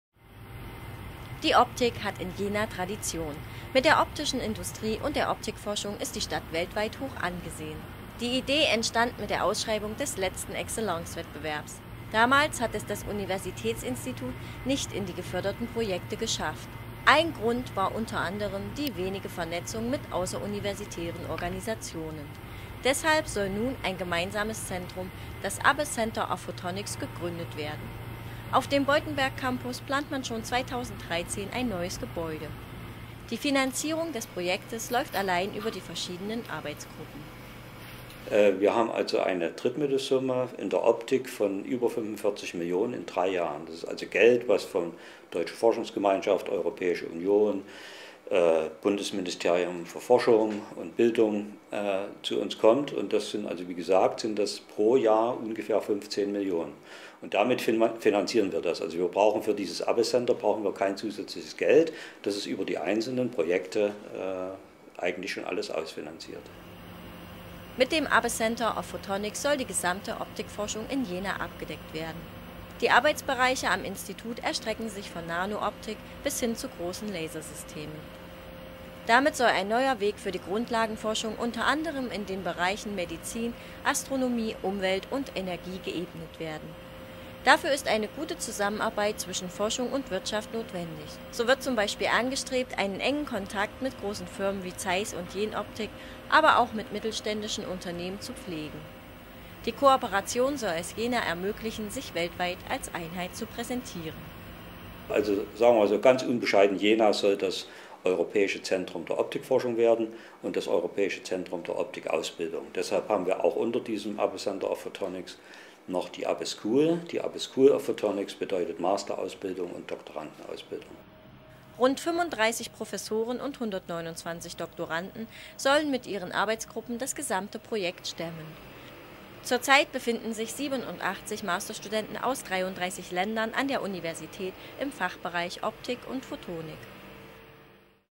Kompetenzzentrum gegründet -- Abbe Center of Photonics soll über Jena hinaus bekannt werden
Die Lichtstadt Jena bekommt Zuwachs im Wissenschaftsbereich. Mit dem „Abbe Center of Phontonics" soll ein neues Institut für die Bereiche Optik und Photonik entstehen. Jena soll damit zukünftig das optische Zentrum in Europa werden.